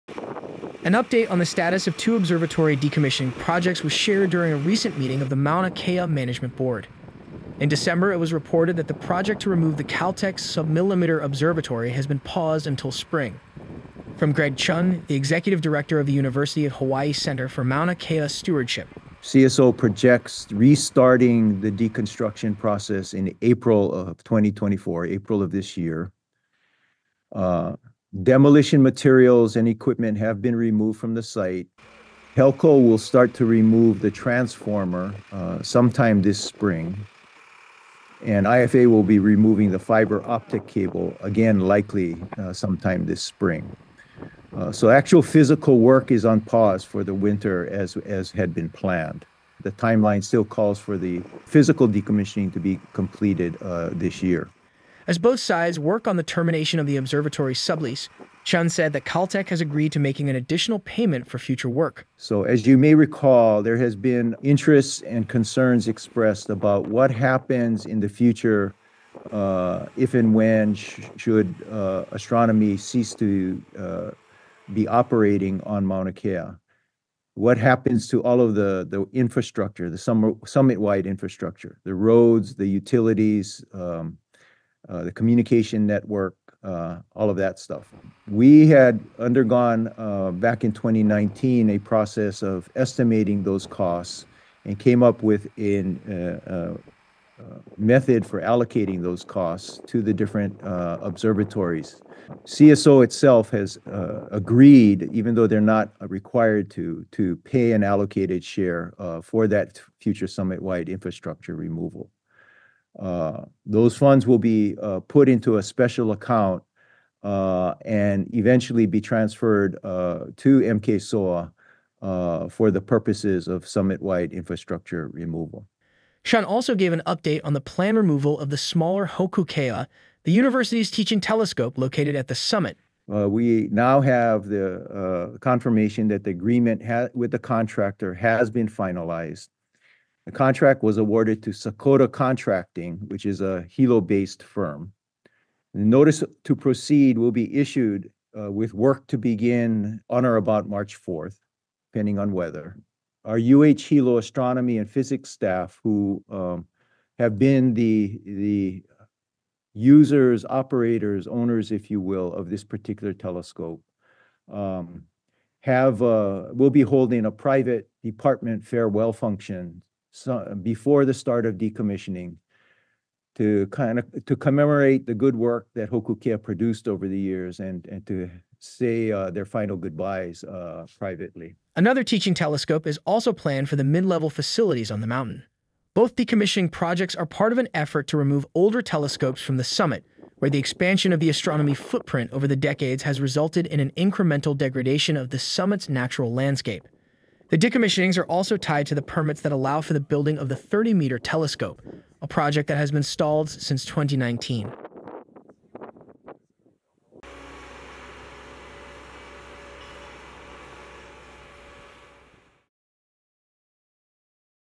Update On Two Maunakea Telescope Removal Projects (Feb 12, 2024)
MAUNAKEA, Hawaiʻi - The University of Hawaiʻi is working on the termination of the CSO sublease, and is preparing for the removal of the Hōkū Keʻa teaching telescope.

A synthesized voice was utilized in the narration for this story.